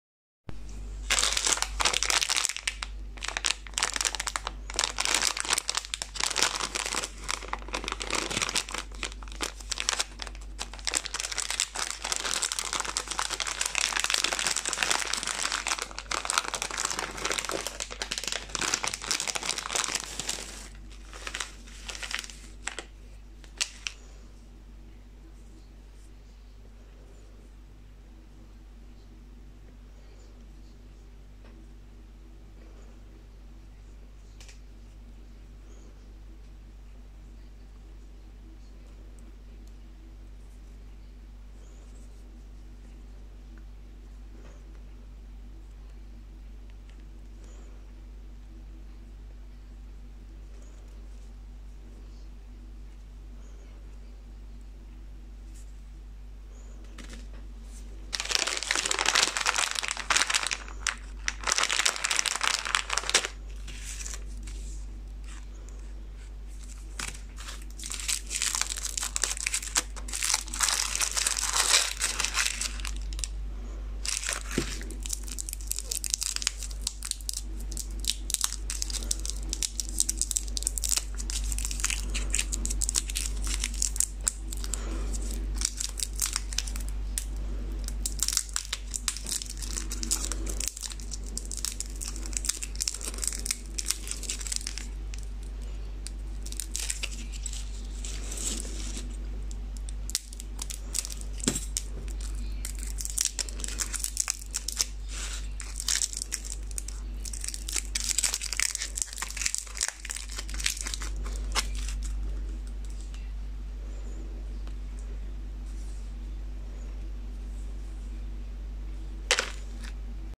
Opening Lollipops ~ Marmalade ~ Satisfying video
Hello to everyone who loves sweets. I will unpack sweets for you every day.